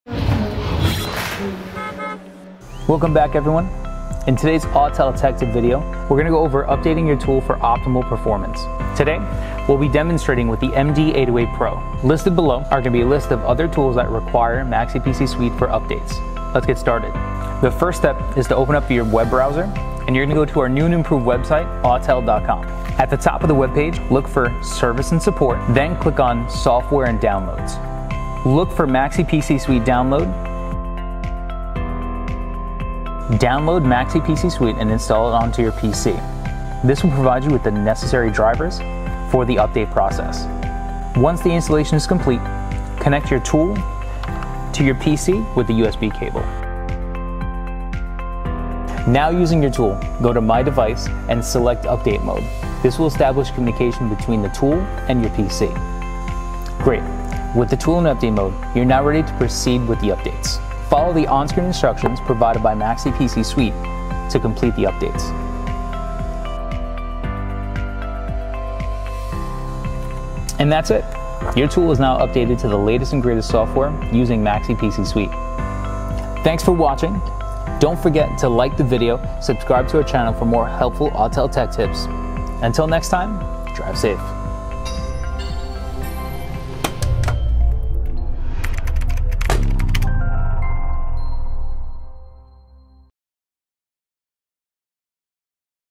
How To Update Autel Tools with Maxi PC Suite by Autel - Tech Tip
Autel Maxi PC Suite allows you to update your VCI, TS508, TS508WF, DIY Products, etc. via your computer. It will also host the drivers for OE software for J2534 platforms. Download software Now!